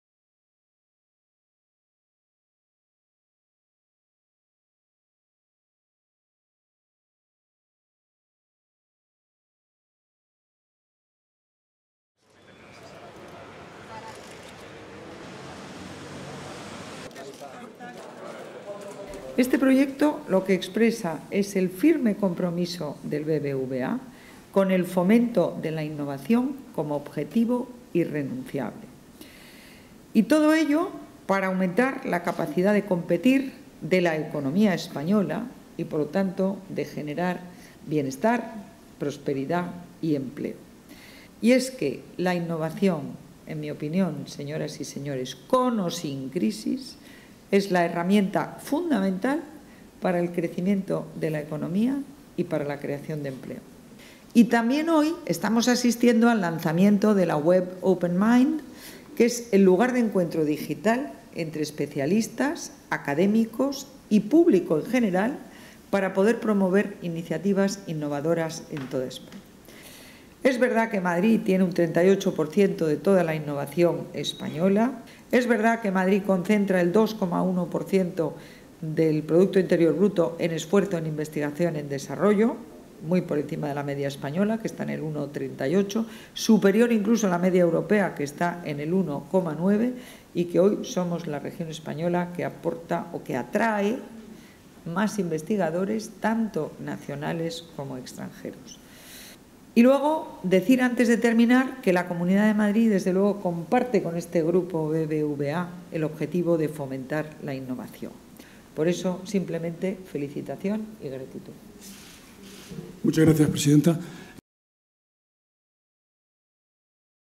Esperanza Aguirre inaugura el Centro de Innovación de BBVA
La Presidenta de la Comunidad de Madrid, Esperanza Aguirre, inaugura el Centro de Innovación de BBVA y asiste a la presentación del libro "Innovación: perspectivas para el siglo XXI" y la web Openmind.